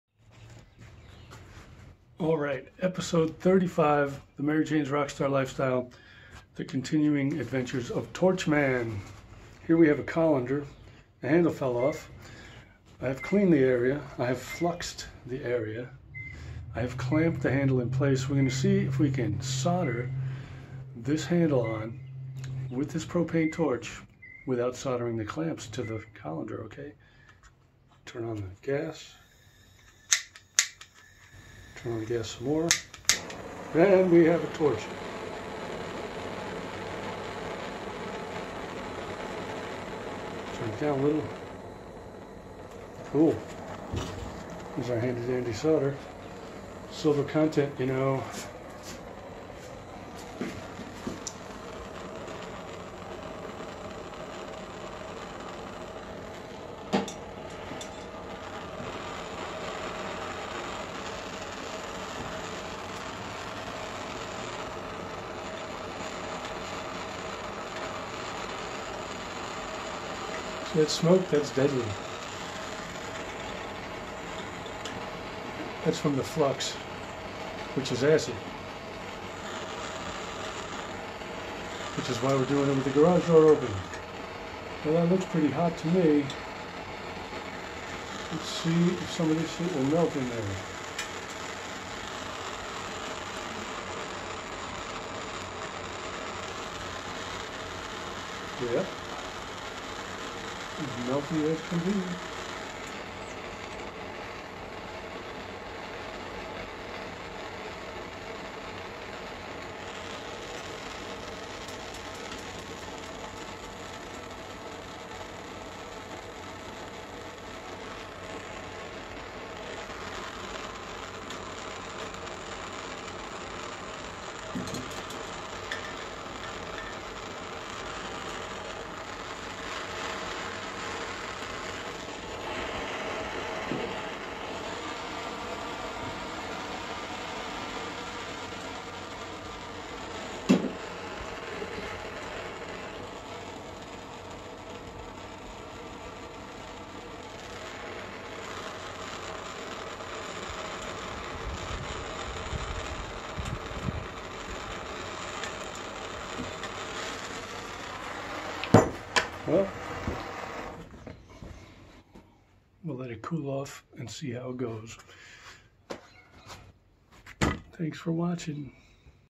Episode 36: The Continuing Adventures of Torch Man
the merry jaynz: rock star lifestyle

After successfully repairing a trombone, Tom's soldering skills are again called upon when one of the handles falls off the colander.